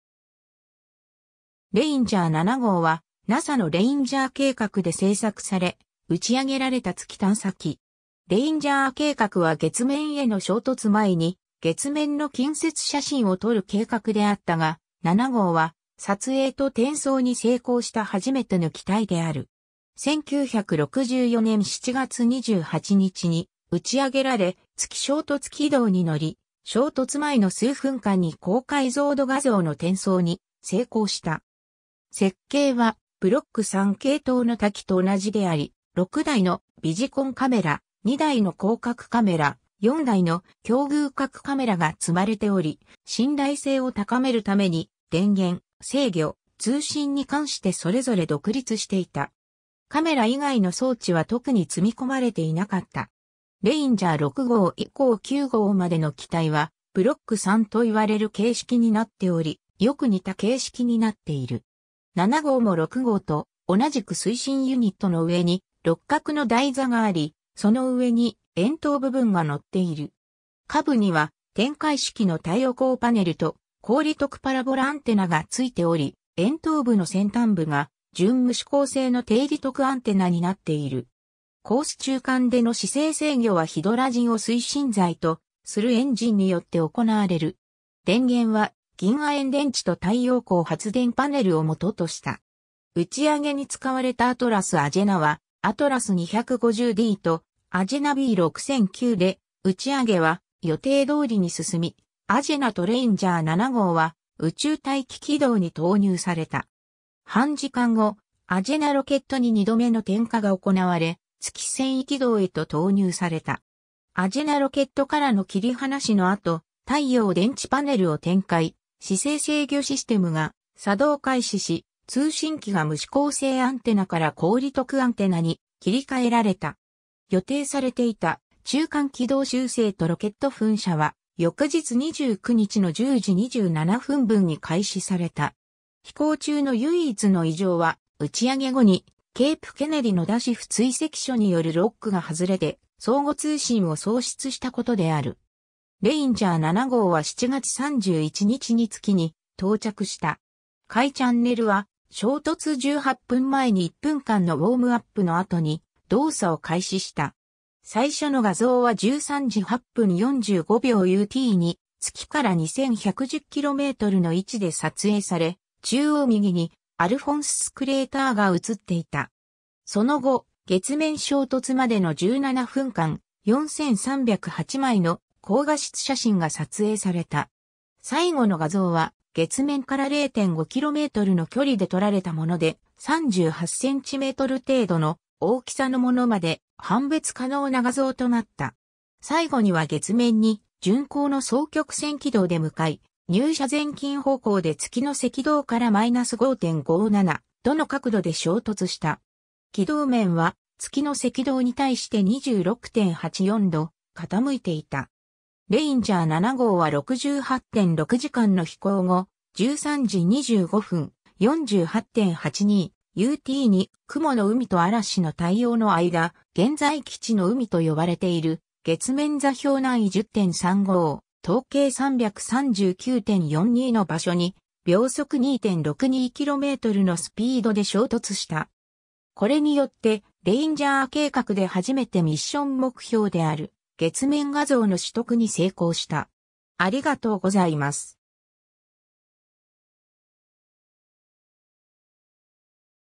レインジャー7号
レインジャー7号(英語: Ranger 7)はNASAのレインジャー計画で製作され、打ち上げられた月探査機。
レインジャー計画は月面への衝突前に月面の近接写真を撮る計画であったが、7号は撮影と転送に成功した初めての機体である。
1964年7月28日に打ち上げられ、月衝突軌道に乗り、衝突前の数分間に高解像度画像の転送に成功した。
設計はブロック3系統の他機と同じであり、6台のビジコンカメラ、2台の広角カメラ、4台の狭隅角カメラが積まれており、信頼性を高めるために電源、制御、通信に関してそれぞれ独立していた。
カメラ以外の装置は特に積み込まれていなかった。
 レインジャー6号以降9号までの機体はブロック3といわれる形式になっており、よく似た形式になっている。
7号も6号と同じく推進ユニットの上に6角の台座があり、その上に円筒部分が載っている。
下部には展開式の太陽光パネルと高利得パラボラアンテナがついており、円筒部の先端部が準無指向性の低利得アンテナになっている。
コース中間での姿勢制御はヒドラジンを推進剤とするエンジンによって行われる。
電源は銀亜鉛電池と太陽光発電パネルを元とした。
 打ち上げに使われたアトラス・アジェナはアトラス250DとアジェナB6009で、打ち上げは予定通りに進み、アジェナとレインジャー7号は宇宙待機軌道に投入された。
半時間後、アジェナロケットに2度目の点火が行われ、月遷移軌道へと投入された。
アジェナロケットからの切り離しの後、太陽電池パネルを展開、姿勢制御システムが作動開始し、通信機が無指向性アンテナから高利得アンテナに切り替えられた。
予定されていた中間軌道修正とロケット噴射は翌日29日の10:27分に開始された。
飛行中の唯一の異常は、打ち上げ後にケープ・ケネディのDSIF追跡所によるロックが外れて相互通信を喪失したことである。
 レインジャー7号は7月31日に月に到着した。
Fチャンネルは衝突18分前に1分間のウォームアップの後に動作を開始した。
最初の画像は13:08:45 UTに月から2110kmの位置で撮影され、中央右にアルフォンススクレーターが写っていた。
その後、月面衝突までの17分間、4308枚の高画質写真が撮影された。
最後の画像は月面から0.5kmの距離で撮られたもので、38cm程度の大きさのものまで判別可能な画像となった。
 最後には月面に順行の双曲線軌道で向かい、入射漸近方向で月の赤道から-5.57度の角度で衝突した。
軌道面は月の赤道に対して26.84度傾いていた。
レインジャー7号は68.6時間の飛行後、13:25:48.82 UTに雲の海と嵐の大洋の間、現在既知の海と呼ばれている月面座標南緯10.35、東経339.42の場所に秒速2.62kmのスピードで衝突した。
（当初の報告では南緯10.63、東経339.34度とされていた。）これによってレインジャー計画で初めてミッション目標である月面画像の取得に成功した。